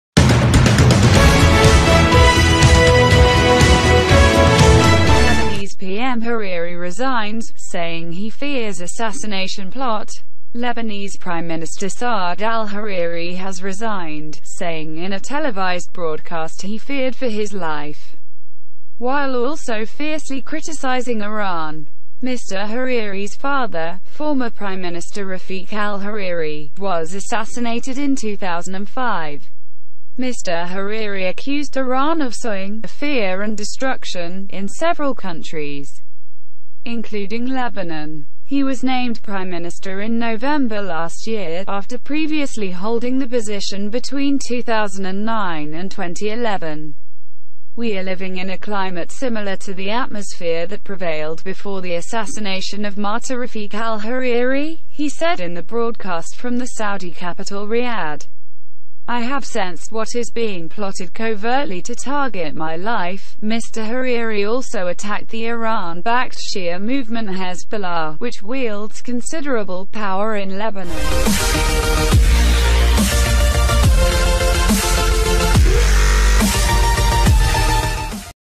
Lebanese PM Hariri resigns, saying he fears assassination plot.
Lebanese Prime Minister Saad al-Hariri has resigned, saying in a televised broadcast he feared for his life, while also fiercely criticising Iran.
Mr Hariri's father, former prime minister Rafik al-Hariri, was assassinated in 2005.
Mr Hariri accused Iran of sowing "fear and destruction" in several countries. including Lebanon.
He was named prime minister in November last year, after previously holding the position between 2009 and 2011.
"We are living in a climate similar to the atmosphere that prevailed before the assassination of martyr Rafik al-Hariri," he said in the broadcast from the Saudi capital Riyadh.
"I have sensed what is being plotted covertly to target my life."
Mr Hariri also attacked the Iran-backed Shia movement Hezbollah, which wields considerable power in Lebanon.